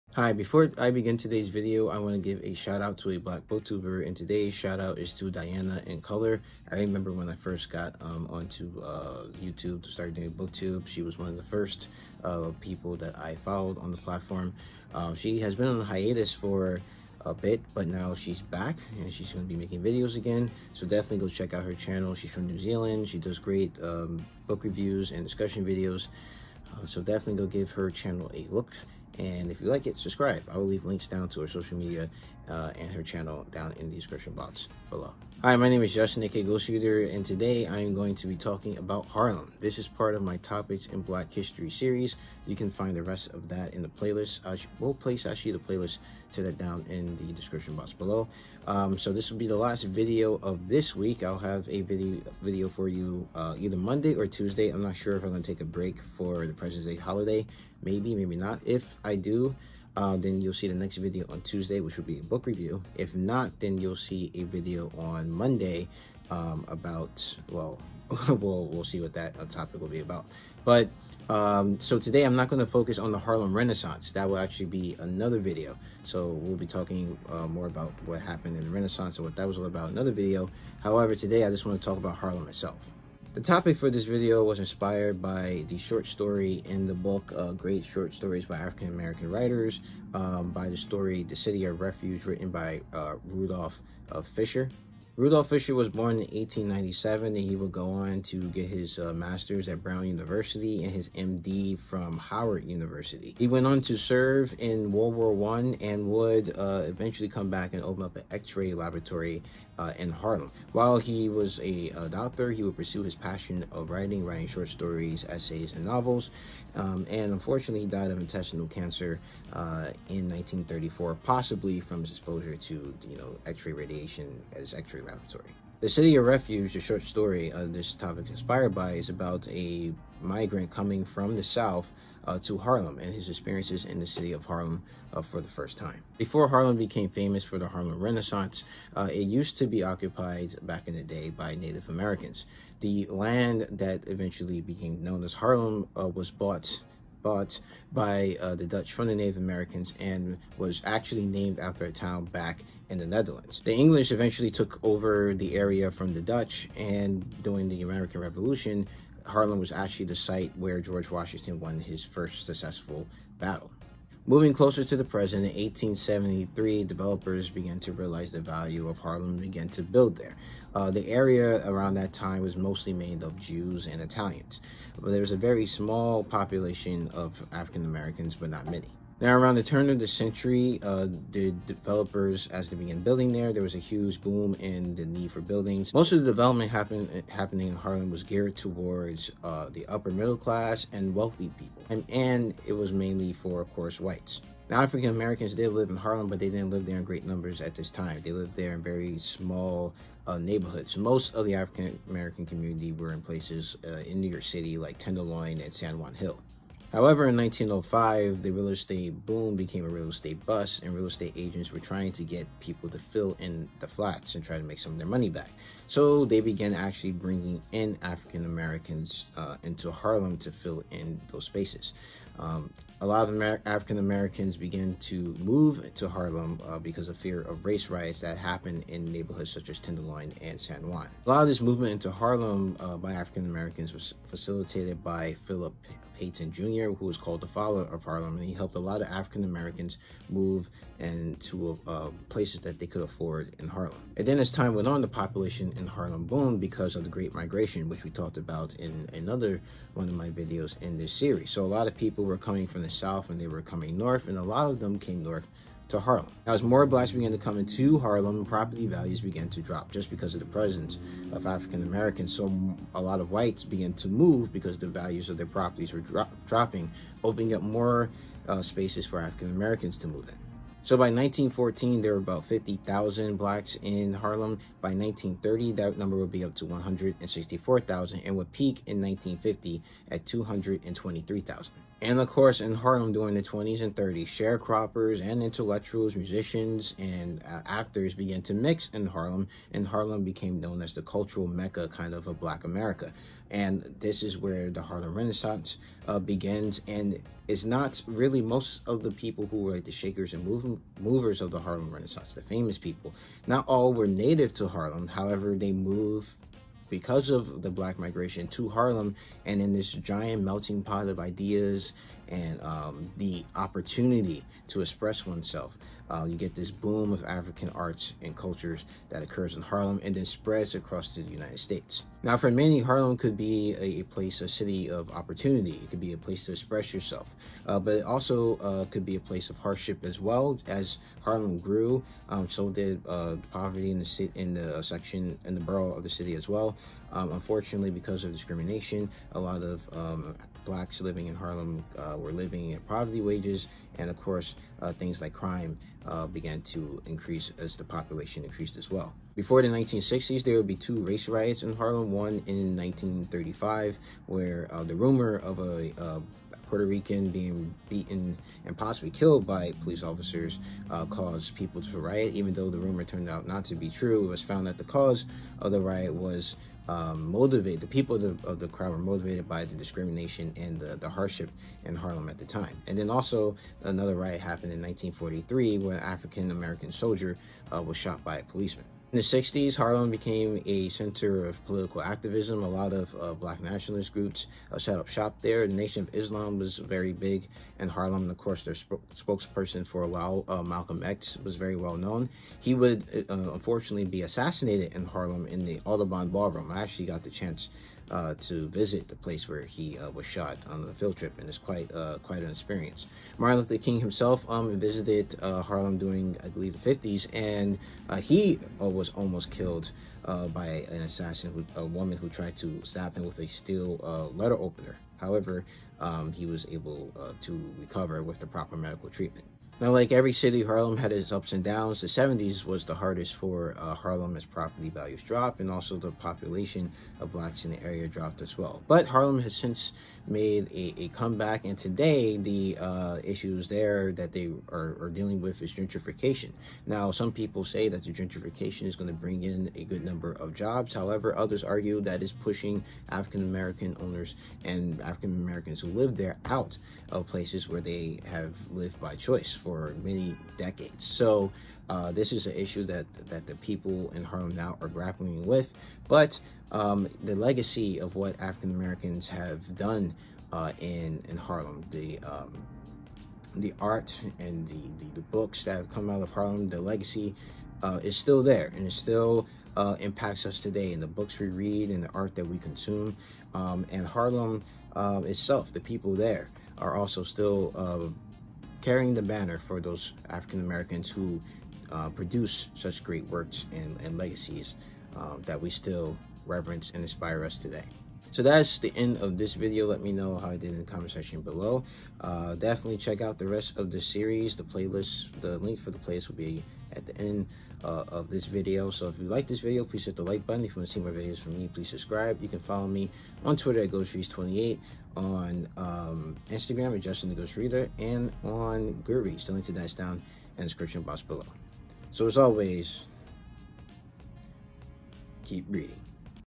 A Brief Look at Harlem: Topics in Black History Series
Hello,

In this video, I take a brief look at the history of Harlem. I will discuss more about Harlem in my video on the Harlem Renaissance.

Black BookTuber Shoutout:
Diana in Colour

This is part of my Topics in Black History Series. Every weekday in the month of February I will be releasing a video related to black history.